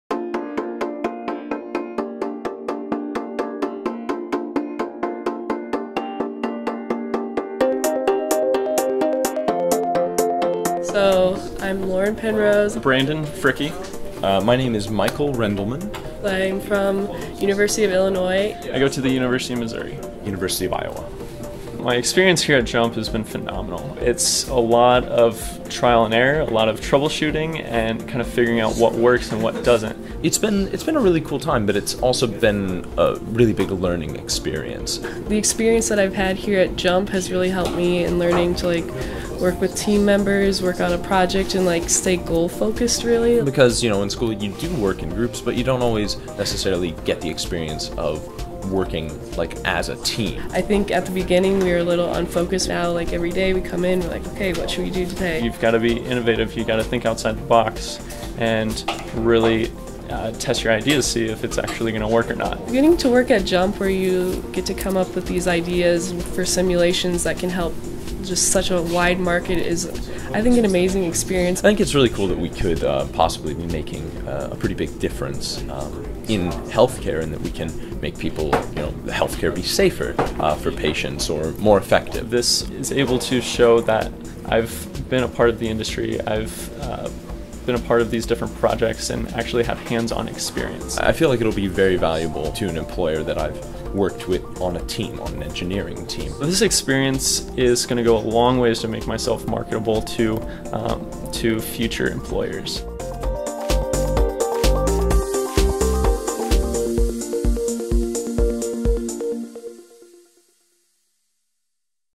Jump Bioengineering Internship Program
Choosing an internship can be tough when you’re in college. You want the prestige of working for a well-known company, but accepting that type of opportunity might mean doing nothing more than menial tasks. Others find places in smaller institutions or those that are less known to ensure they get hands-on experience.

At Jump, interns get the best of both worlds. We’re linked to a large health care system and our interns, with the help of experienced mentors, get to work on helping us solve serious problems facing the industry.

Michael Rendleman, Brandon Fricke, and Lauren Penrose participated in the 2015 internship program and shared their experiences.